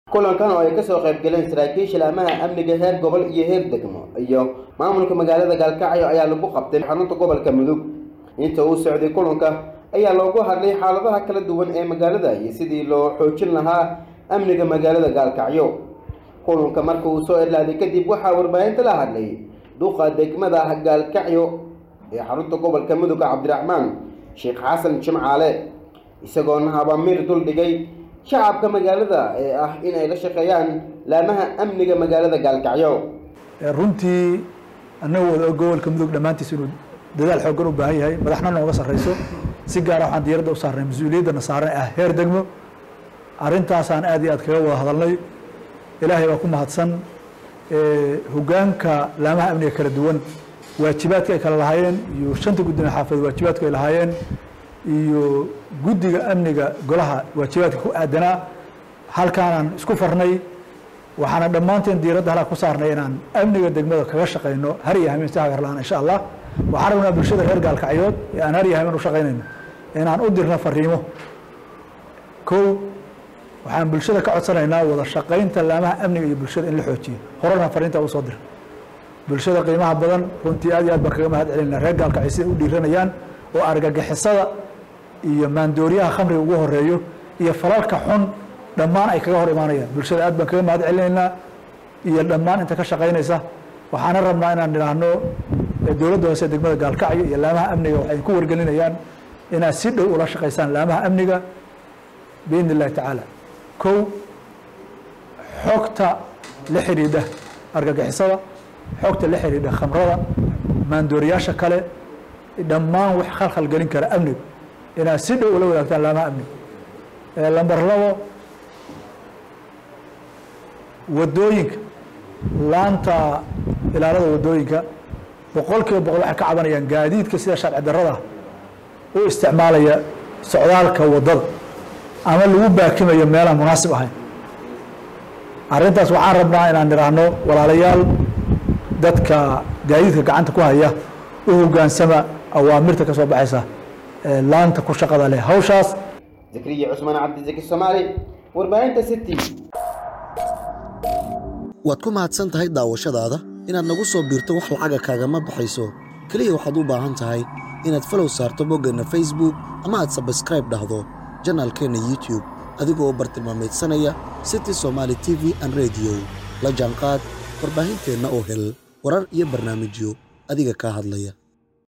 Mamulka Magaalada Gaalkacyo oo laamaha Amniga Magaaladaasi kala hadlay Sugida Amniga Degmada
Mamulka Magaalada Gaalkacyo oo laamaha Amniga Magaaladaasi kala hadlay Sugida Amniga Degmada By Zeki Somali